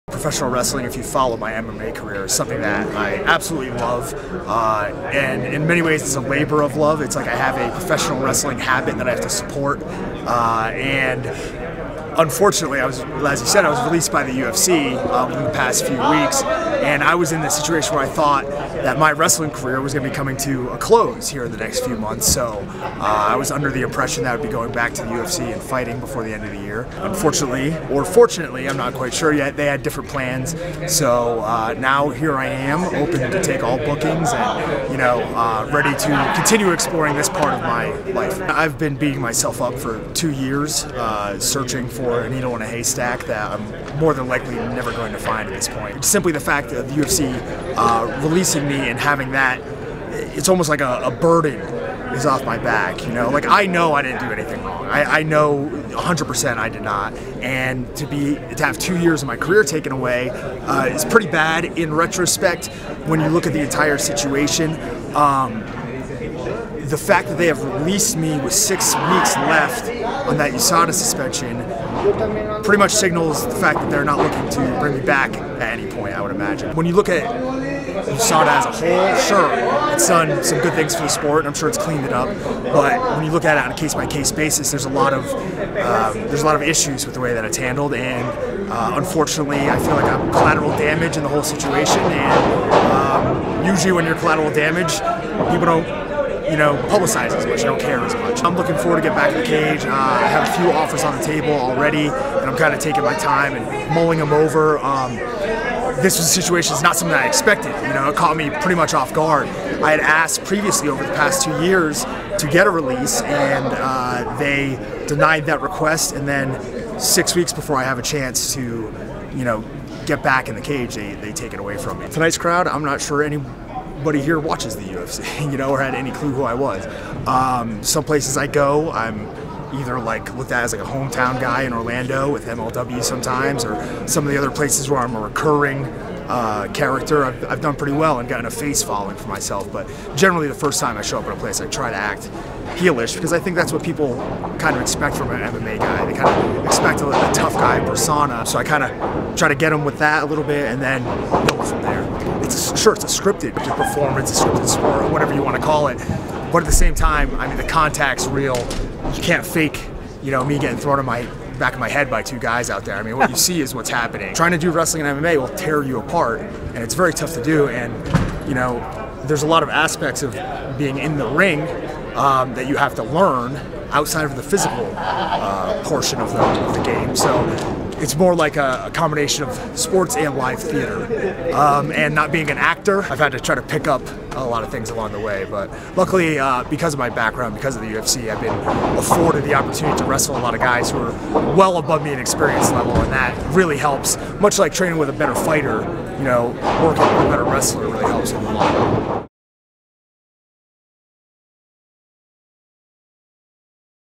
Tom Lawlor looking for MMA bouts while honing his pro wrestling skills
Following his UFC release, Tom Lawlor wrestled at Future Legend's Expo Lucha in Las Vegas on Aug. 31. Though he has no intention of ending his MMA career, the former UFC light heavyweight and middleweight said he'll continue to pursue his options in pro wrestling while he looks for a new MMA promotion to sign him.